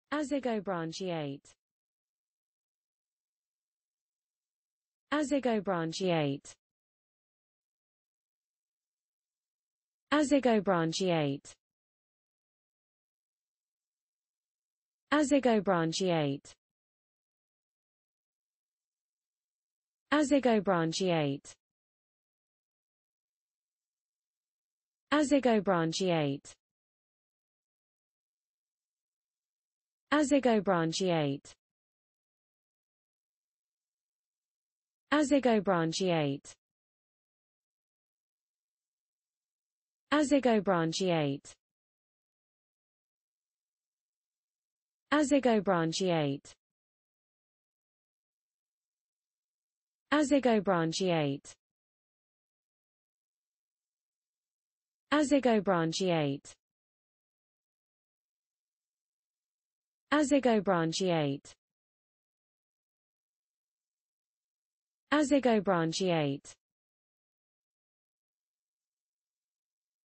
azygobranchiate: How to pronounce azygobranchiate in english (correct!).Start with A. Learn from m
Follow me!Learn from me how to pronounce the word azygobranchiate in english.This video shows you how to pronounce the word azygobranchiate in english.

azygobranchiate